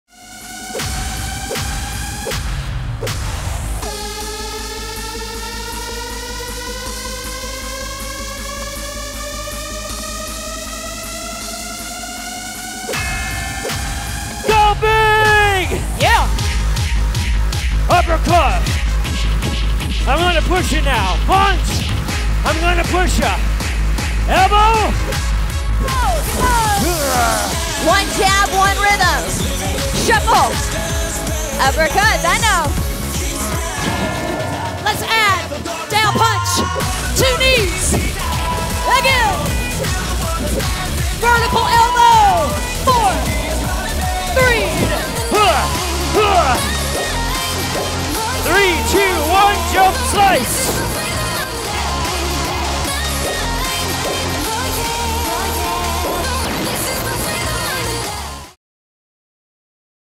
Group Fight APR21 Trailer
Group Fight® is a gripping hour that builds cardio fitness, total-body strength, and coordination. Combine the hottest, adrenaline-fueled MMA movements from the boxing ring to the fighting cage with cutting-edge exercises from outside the octagon. Thrilling music and motivational coaching will get you fighting fit. FIGHT FOR IT!